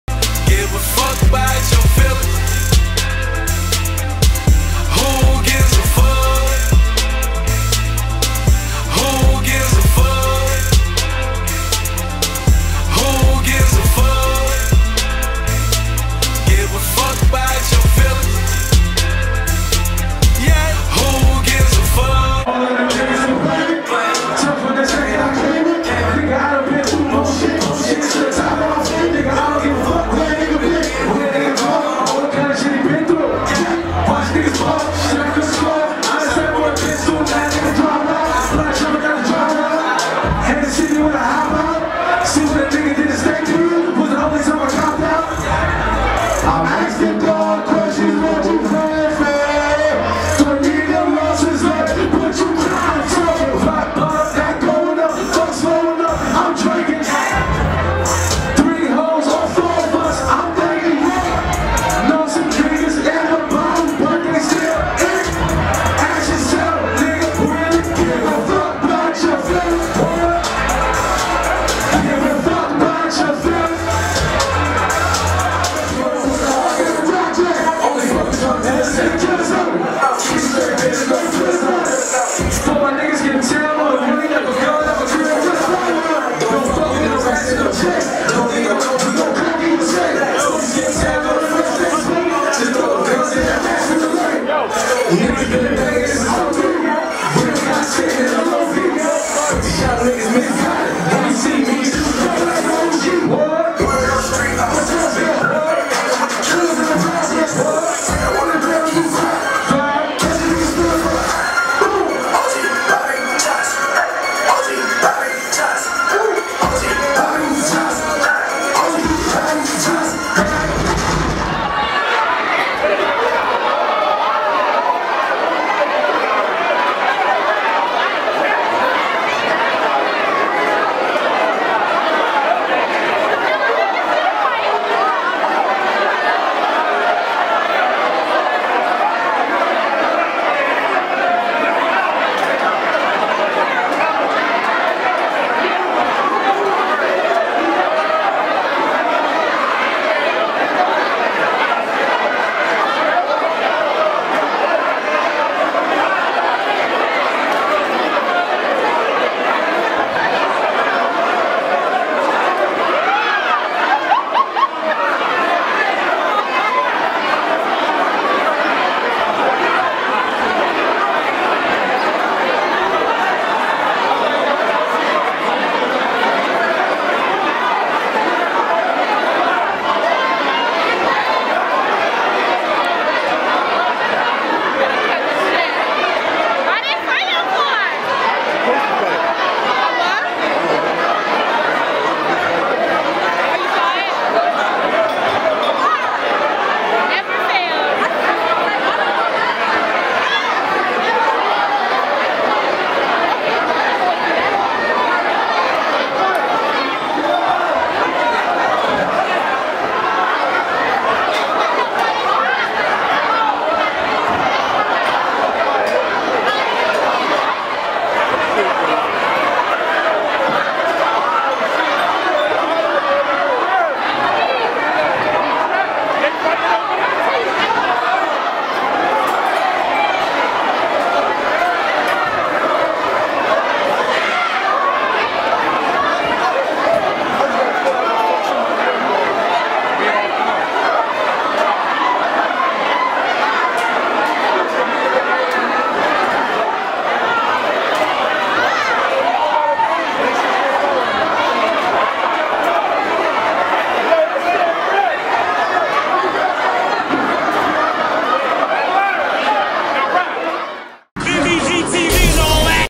Chinx Drugz Live at Club Pyur & Fights Break Out
CHINX DRUGZ LIVE @ CLUB PYUR IN AND FIGHT BREAKS OUT!